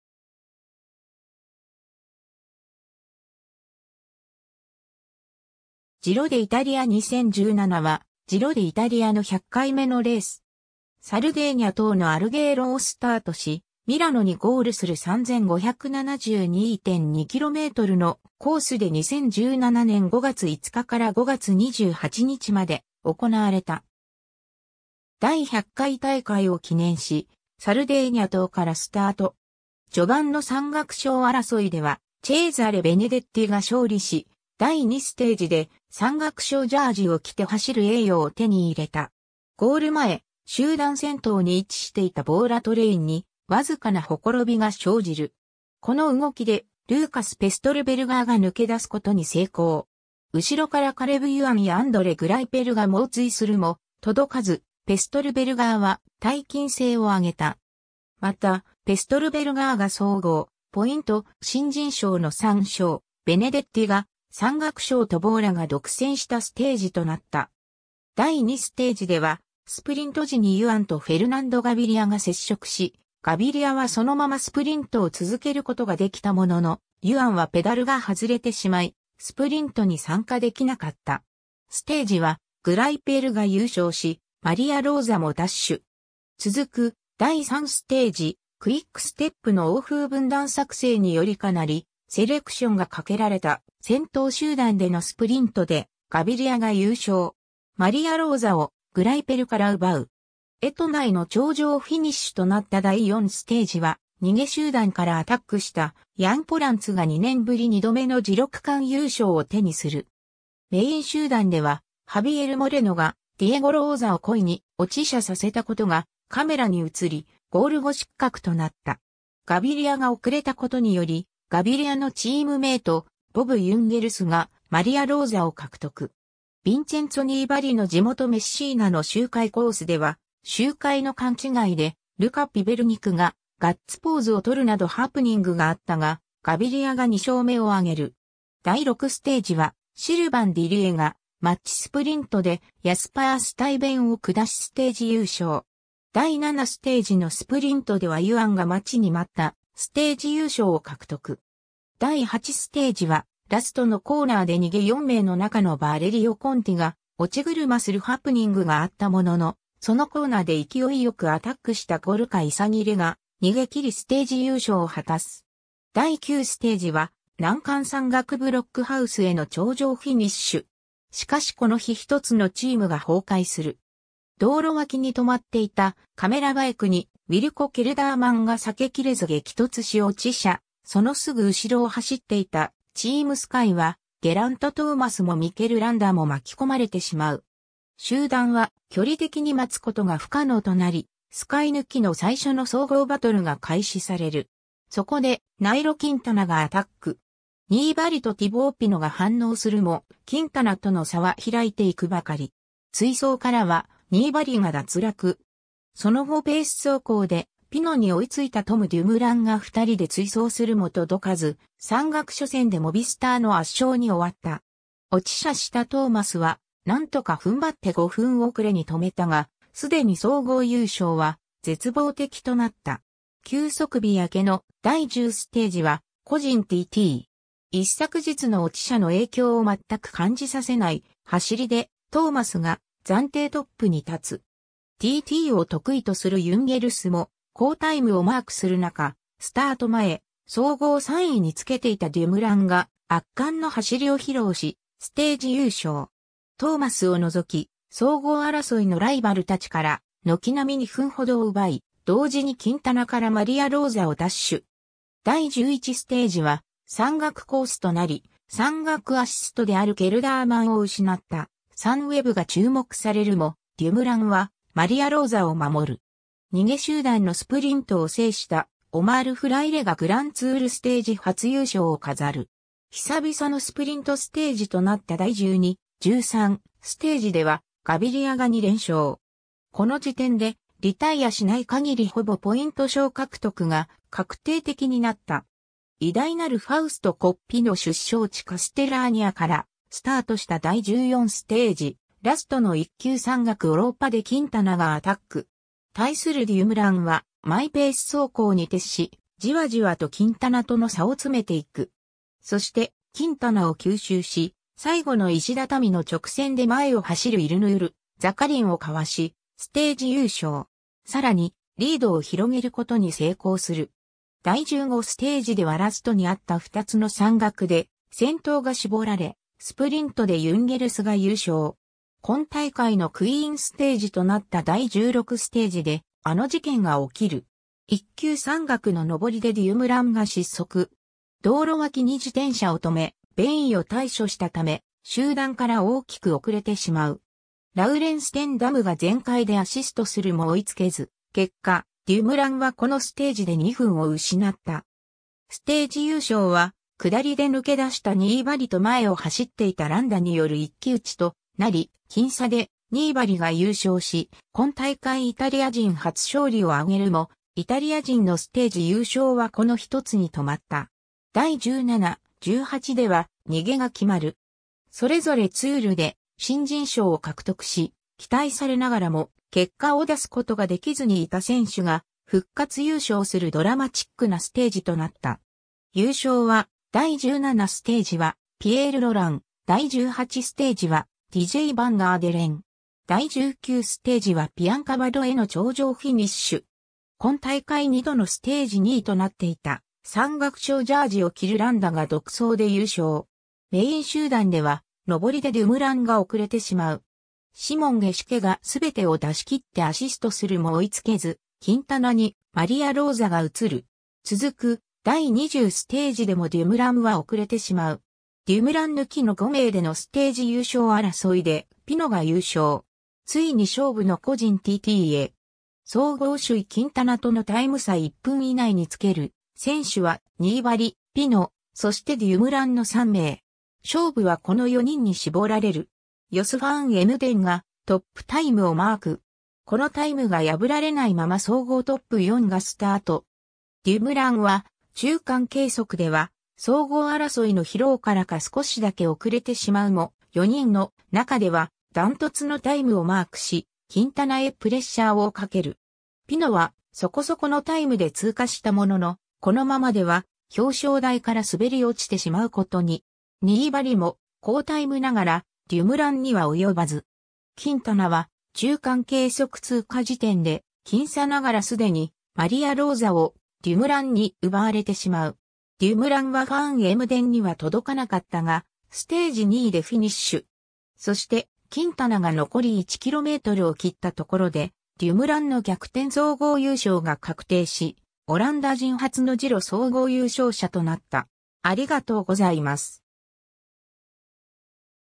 ジロ・デ・イタリア2017
ジロ・デ・イタリア
ジロ・デ・イタリア2017 (Giro d'Italia 2017) は、ジロ・デ・イタリアの100回目のレース。
サルデーニャ島のアルゲーロをスタートし、ミラノにゴールする3,572.2kmのコースで2017年5月5日から5月28日まで行なわれた。
 第100回大会を記念し、サルデーニャ島からスタート。
序盤の山岳賞争いではチェーザレ・ベネデッティ(ボーラ＝ハンスグローエ)が勝利し、第2ステージで山岳賞ジャージを着て走る栄誉を手に入れた。
 ゴール前、集団先頭に位置していたボーラトレインに僅かな綻びが生じる。
この動きでルーカス・ペストルベルガー(ボーラ＝ハンスグローエ)が抜け出すことに成功。
後ろからカレブ・ユアン(オリカ・スコット)やアンドレ・グライペル(ロット・ソウダル)が猛追するも届かず、ペストルベルガーは大金星をあげた。
また、ペストルベルガーが総合、ポイント、新人賞の3賞、ベネデッティが山岳賞とボーラが独占したステージとなった。
 第2ステージではスプリント時にユアンとフェルナンド・ガビリア(クイックステップ・フロアーズ)が接触し、ガビリアはそのままスプリントを続けることができたものの、ユアンはペダルが外れてしまい、スプリントに参加できなかった。
ステージはグライペルが優勝し、マリア・ローザも奪取。
続く第3ステージ、クイックステップの横風分断作成によりかなりセレクションが掛けられた先頭集団でのスプリントでガビリアが優勝。
マリア・ローザをグライペルから奪う。
 エトナへの頂上フィニッシュとなった第4ステージは逃げ集団からアタックしたヤン・ポランツ(UAEチーム・エミレーツ)が2年ぶり2度目のジロ区間優勝を手にする。
メイン集団ではハビエル・モレノ(バーレーン・メリダ)がディエゴ・ローザ(チーム・スカイ)を故意に落車させたことがカメラに映り、ゴール後失格となった。
ガビリアが遅れたことによりガビリアのチームメート、ボブ・ユンゲルス(クイックステップ・フロアーズ)がマリア・ローザを獲得。
 ヴィンチェンツォ・ニーバリ(バーレーン・メリダ)の地元メッシーナの周回コースでは周回の勘違いでルカ・ピベルニク(バーレーン・メリダ)がガッツポーズをとるなどハプニングがあったが、ガビリアが2勝目をあげる。
 第6ステージはシルヴァン・ディリエ(BMCレーシング)がマッチスプリントでヤスパー・スタイヴェン(トレック・セガフレード)を下しステージ優勝。
第7ステージのスプリントではユアンが待ちに待ったステージ優勝を獲得。
 第8ステージはラストのコーナーで逃げ4名のなかのヴァレリオ・コンティ(UAEチーム・エミレーツ)が落車するハプニングがあったものの、そのコーナーで勢いよくアタックしたゴルカ・イサギレ(モビスター)が逃げ切りステージ優勝を果たす。
 第9ステージは難関山岳ブロックハウス（英語版）への頂上フィニッシュ。
 しかしこの日1つのチームが崩壊する。
 道路脇に止まっていたカメラバイクに、ウィルコ・ケルダーマン(サンウェブ)が避けきれず激突し落車、そのすぐ後ろを走っていたチーム・スカイはゲラント・トーマス(チーム・スカイ)もミケル・ランダ(チーム・スカイ)も巻き込まれてしまう。
 集団は距離的に待つことが不可能となり、スカイ抜きの最初の総合バトルが開始される。
そこでナイロ・キンタナ(モビスター)がアタック。
ニーバリとティボー・ピノ(FDJ)が反応するもキンタナとの差は開いていくばかり。
追走からはニーバリが脱落。
その後ペース走行でピノに追いついたトム・デュムラン(サンウェブ)が2人で追走するも届かず、山岳初戦でモビスターの圧勝に終わった。
落車したトーマスは何とか踏ん張って5分遅れに留めたがすでに総合優勝は絶望的となった。
 休息日明けの第10ステージは個人TT。
一昨日の落車の影響を全く感じさせない走りでトーマスが暫定トップに立つ。
TTを得意とするユンゲルスも好タイムをマークするなか、スタート前総合3位につけていたデュムランが圧巻の走りを披露し、ステージ優勝。
トーマスを除き、総合争いのライバルたちから軒並み2分ほどを奪い、同時にキンタナからマリア・ローザを奪取。
 第11ステージは山岳コースとなり、山岳アシストであるケルダーマンを失ったサンウェブが注目されるも、デュムランはマリア・ローザを守る。
逃げ集団のスプリントを制したオマール・フラ...